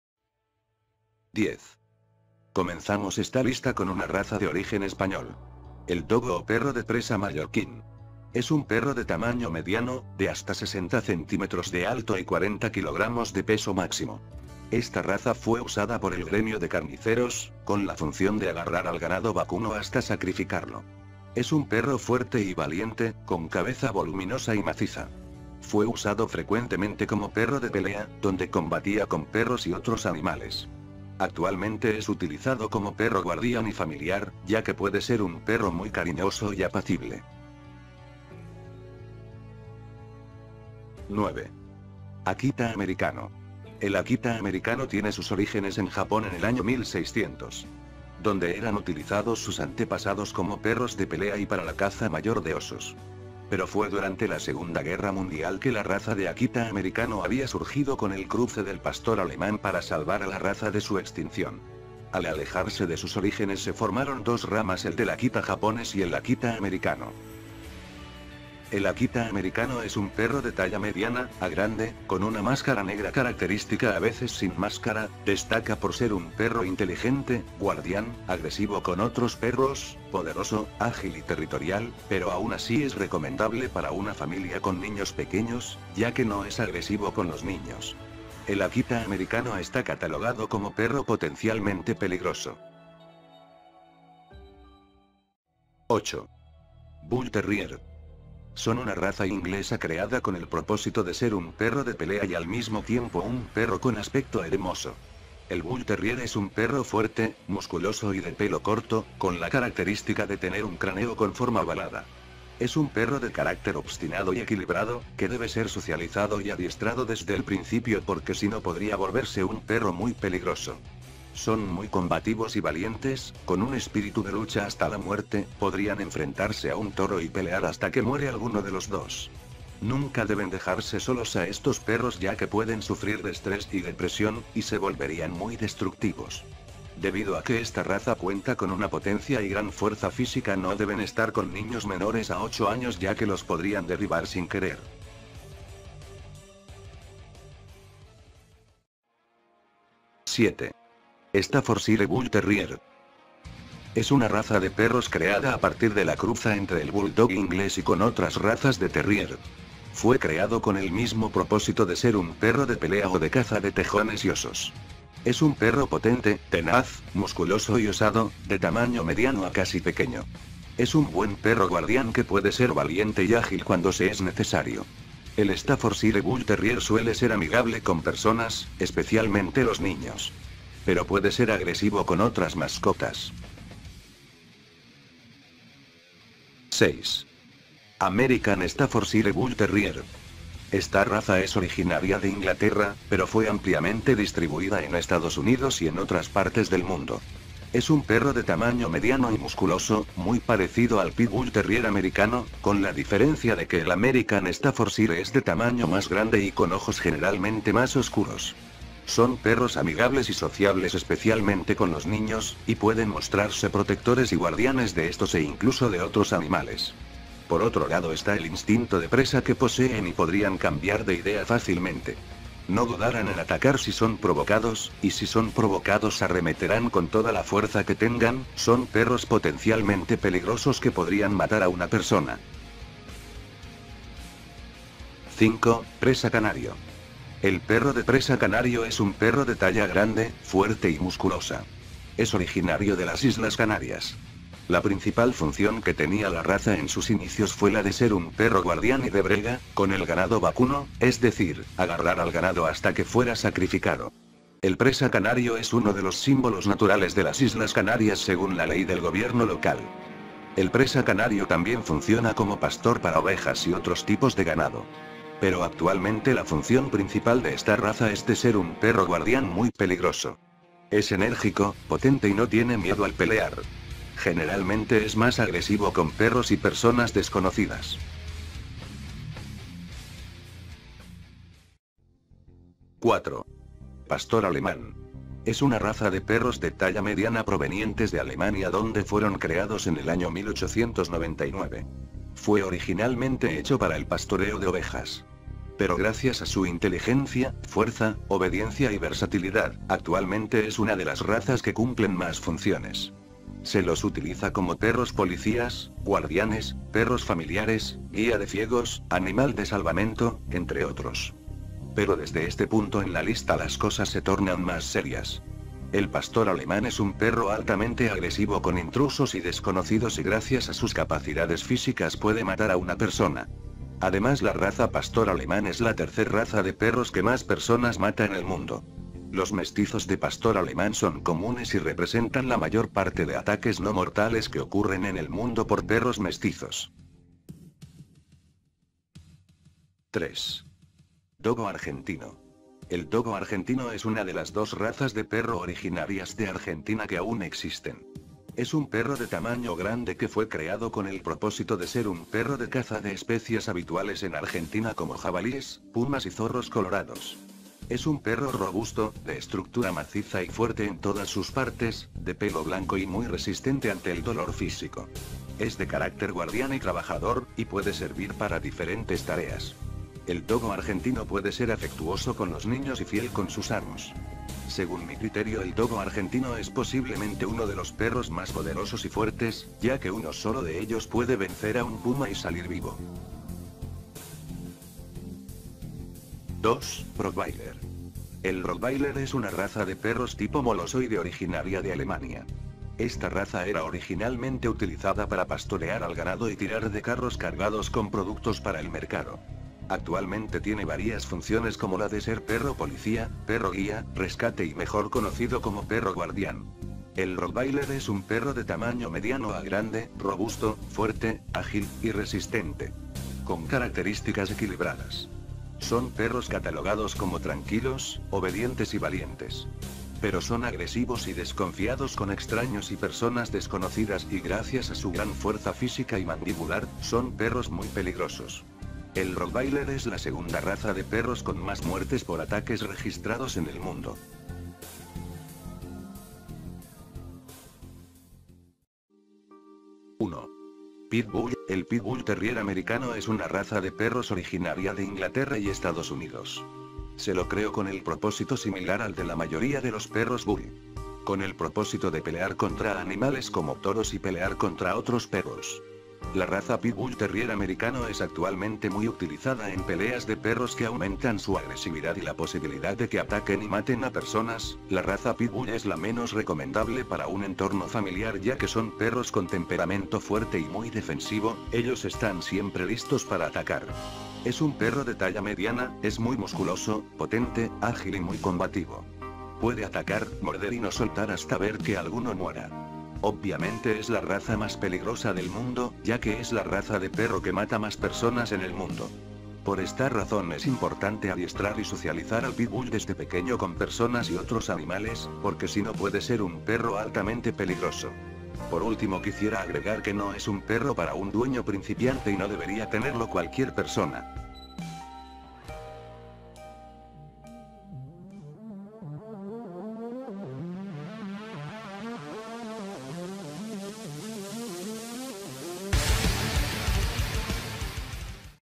LAS 10 RAZAS DE PERROS MÁS PELIGROSAS DEL MUNDO
Hola bienvenido a DOS CAMINOS.
Hola suscriptores y gente de youtube. Esta vez les presentare las 10 razas de perros más peligrosas del mundo, ya que poseen características físicas y psicológicas que los hacen perros de gran potencia y capacidad para matar a una persona, pero esto no quiere decir que sean peligrosos por naturaleza, un perro actúa conforme como se lo críe y eduque por esa razón si se lo maltrata y  utilizan para luchar o pelear, es más propenso a matar o atacar a una persona o animal. Por eso es conveniente adiestrar correctamente y socializar a un perro desde pequeño. Pero también hay que tener en cuenta el factor de la raza de perro. Hay algunas como los Pit Bull y otras muy distintas como el Golden Retriver con sus cualidades especiales. Pero cualquier perro ya sea de raza o mestizo puede ser una buena mascota o un amigo inseparable solo si sabes como tratarlo bien, educarlo y quererlo.

Espero que les guste el vídeo. Denle un me gusta y comenten si ha sido de su agrado.